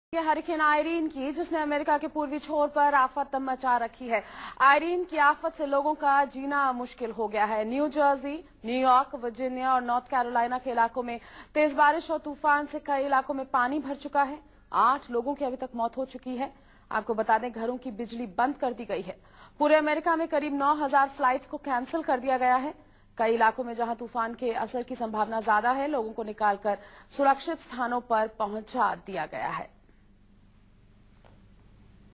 Tropical storm Irene weakens as it sweeps into New York
Tropical Storm Irene unleashed furious wind and rain on New York on Sunday and sent seawater surging into the Manhattan streets.